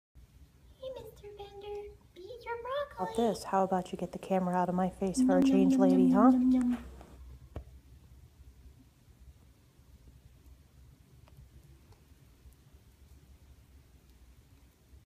Mr. Bender does NOT like the camera!! (pet rat videos)
All Mr. Bender wants to do is eat his broccoli and he tells me like it is!